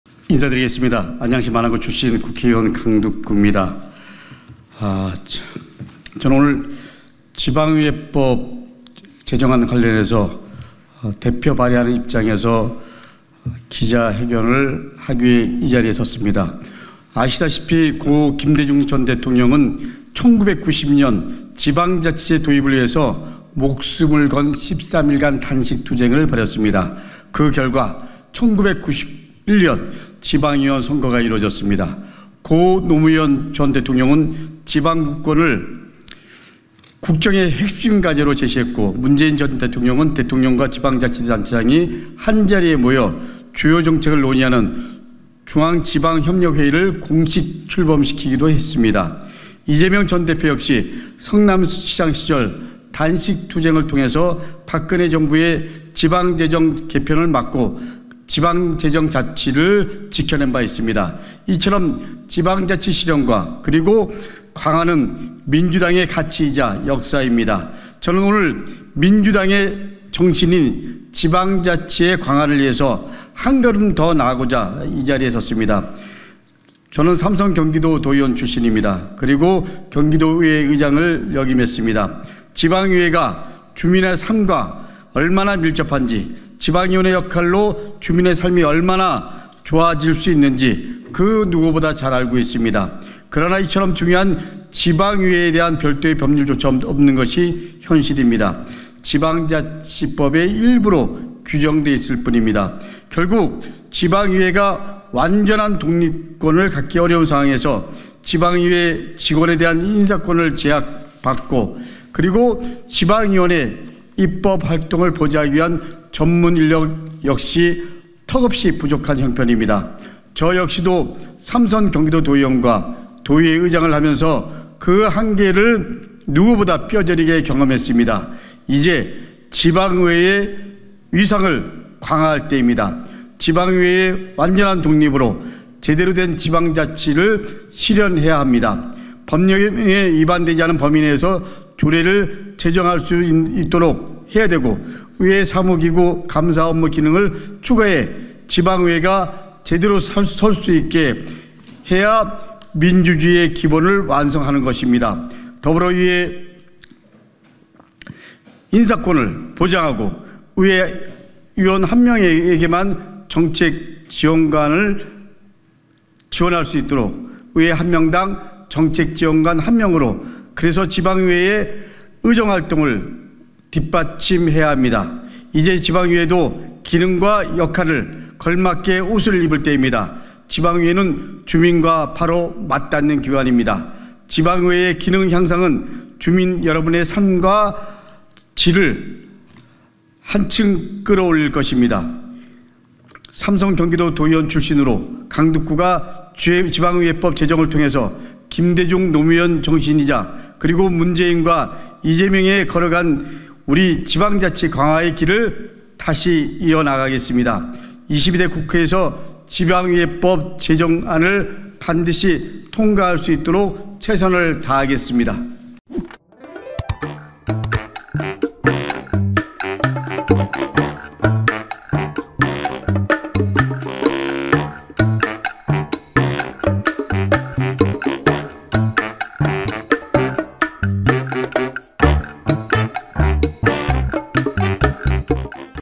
지방의회 위상 강화 '지방의회법'... '완전한 독립' 요구한 민주당 강득구
더불어민주당 강득구 의원이 24년 7월 24일 국회 소통관 기자회견장에서 기자회견을 하고 있다.